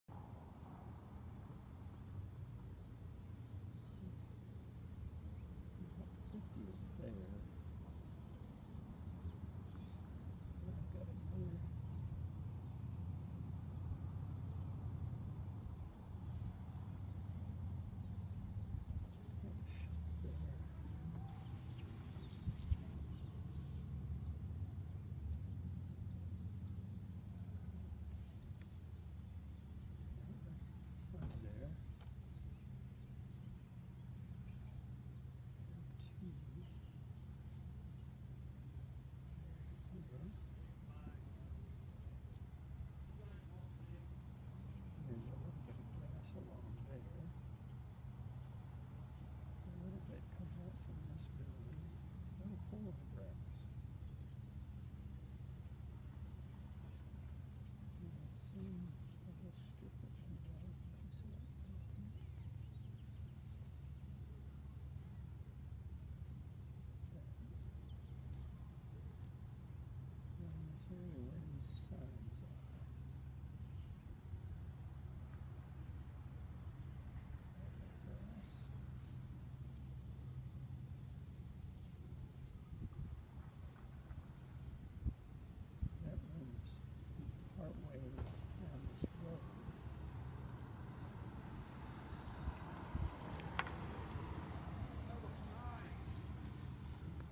Painting "At Sandoval" 4
A short video showing the process for location painting "At Sandoval" painted morning of 5/5/2018 in Sandoval, IL.